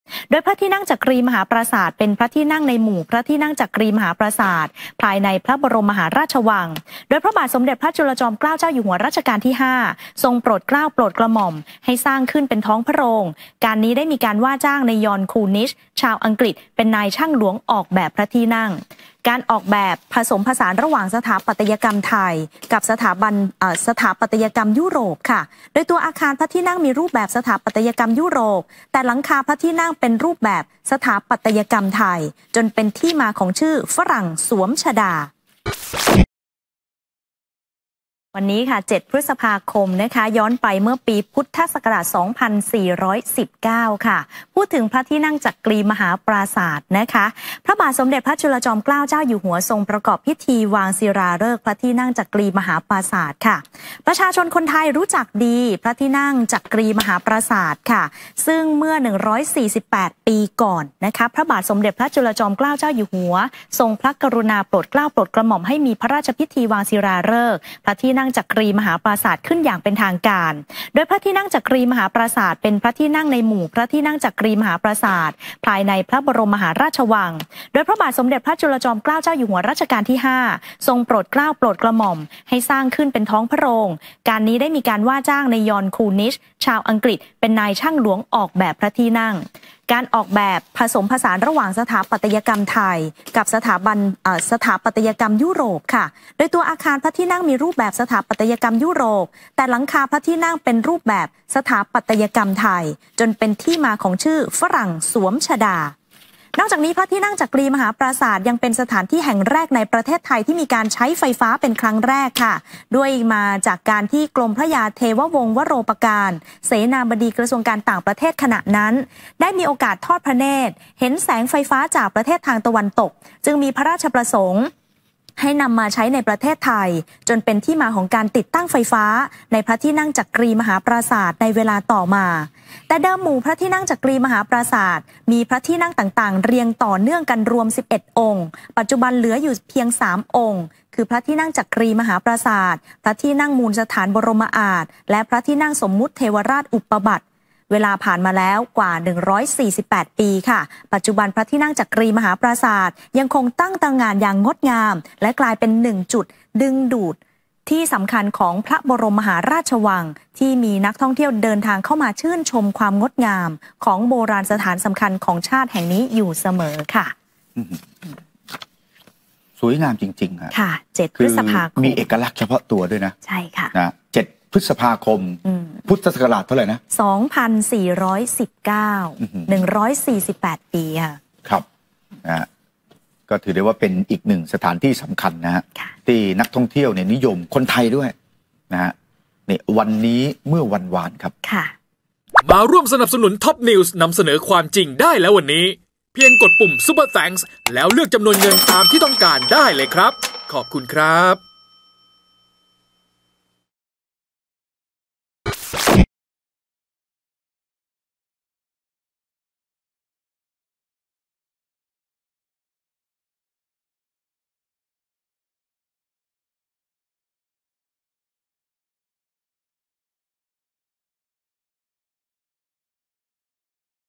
"พระที่นั่งจักรีมหาปราสาท" ร.5 ทรงวางศิลาฤกษ์ 7 พฤษภาคม พ.ศ. 2419
วันนี้เมื่อวันวาน "พระที่นั่งจักรีมหาปราสาท" ร.5 ทรงวางศิลาฤกษ์ 7 พฤษภาคม พ.ศ. 2419